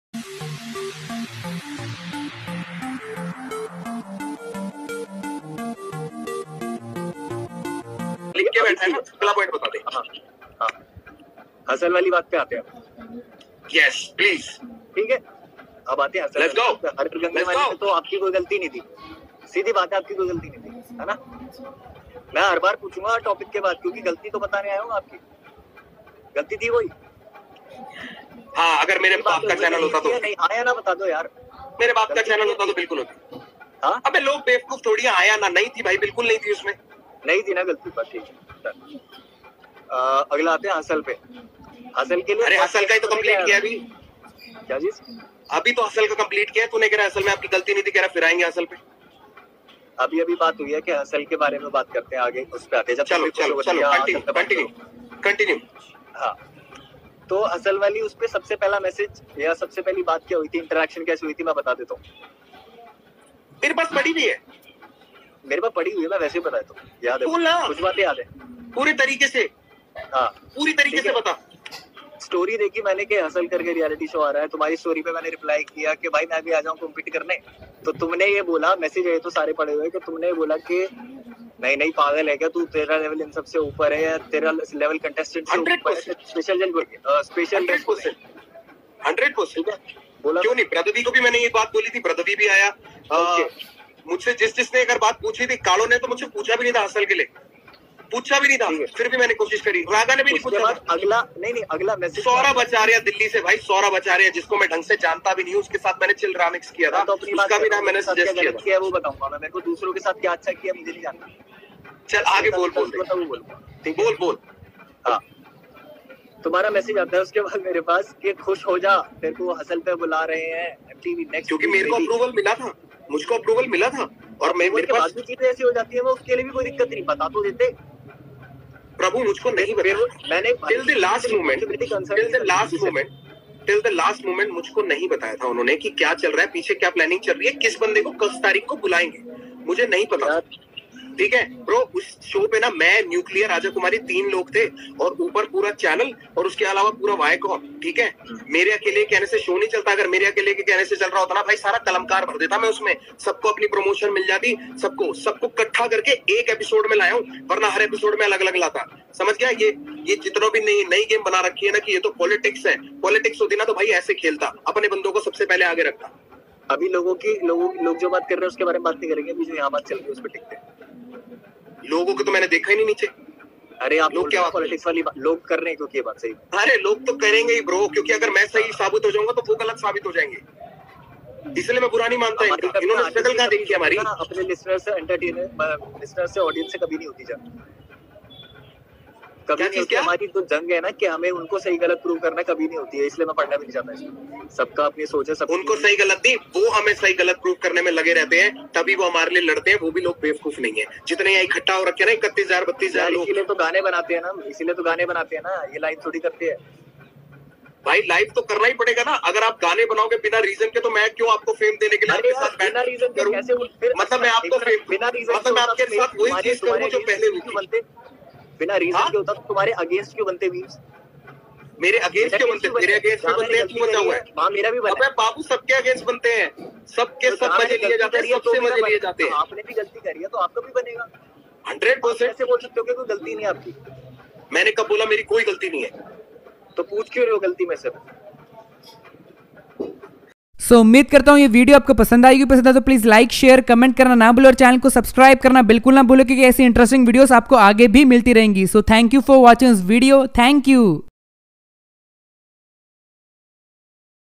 Raftaar Angry On Muhfaad | Mtv Hustle Issue Reveals | Muhfaad Reply To Hard Reply To Raftaar !
Raftaar reply to muhfaad on mtv hustle issue sikander kahlon !

Instagram user name : harish3414